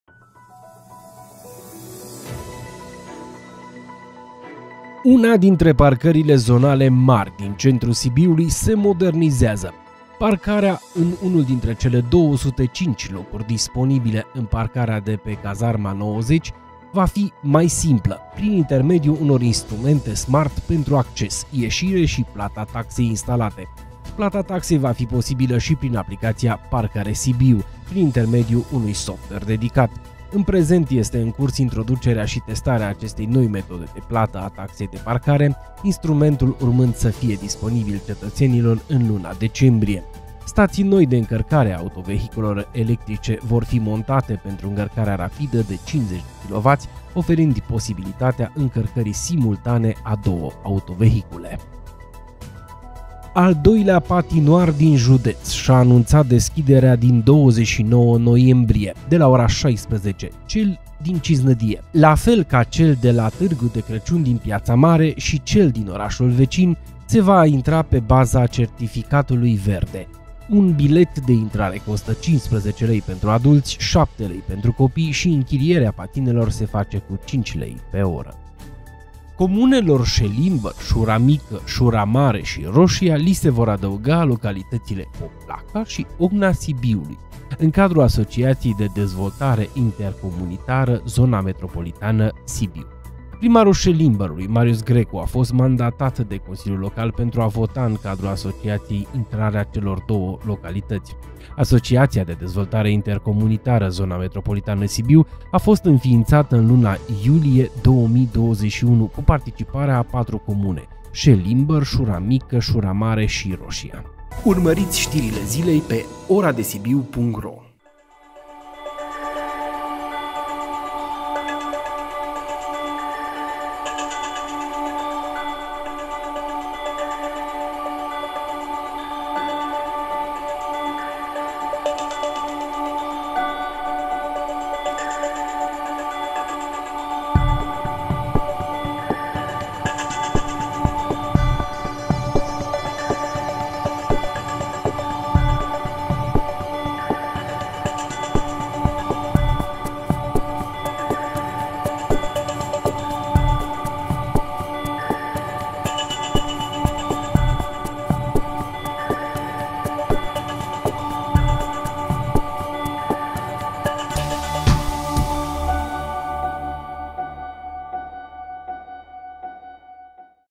Parcarea Cazarma 90 devine smart – Se montează senzori de recunoaştere a numerelor de înmatriculare
Parcarea Cazarma 90 devine smart – Se montează senzori de recunoaştere a numerelor de înmatriculare şi alte două știri de luni 29 noiembrie